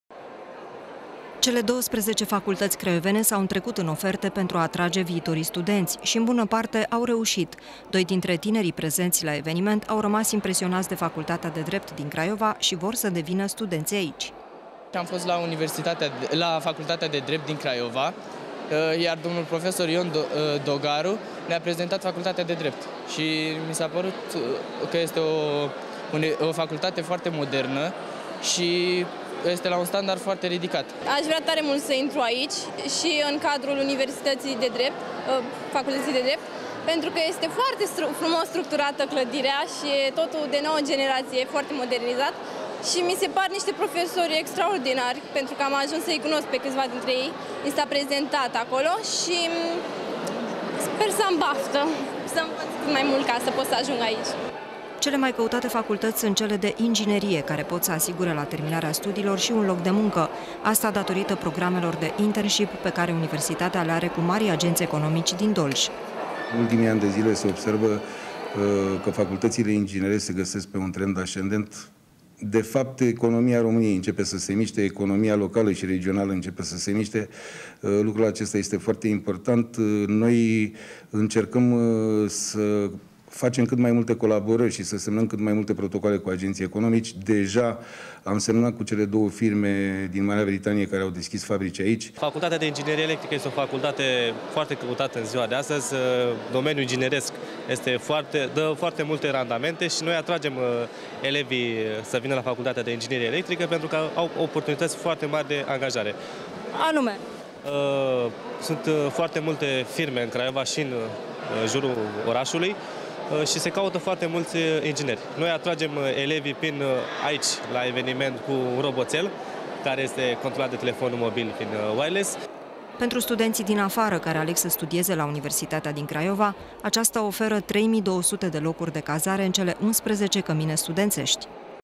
PORŢI DESCHISE LA UNIVERSITATEA DIN CRAIOVA
2.800 de locuri la licenţă la buget, 1600 la master şi 5 noi programe de studii de masterat în anul scolar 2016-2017- aceasta este oferta educaţională prezentată de Universitatea din Craiova viitorilor studenţi. Tinerii liceeni din anii terminali au fost prezenţi în număr mare la Ziua Porţilor Deschise, pentru a vedea oferta facultăţilor craiovene.